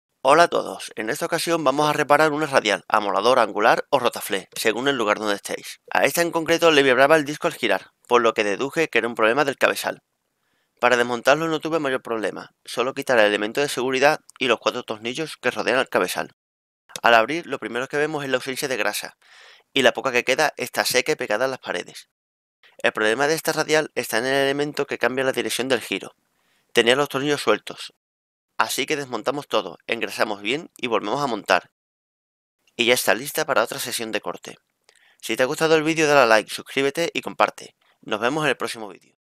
Raparación de rotaflé o radial
Pues el problema me vino porque el disco empezó a vibrar, entonces me decidí a desmontarlo para ver que pasaba. Es una reparación sencilla, con un par de destornilladores es suficiente.

Si tenéis cualquier duda escribirla en los comentarios y os responderé lo antes posible.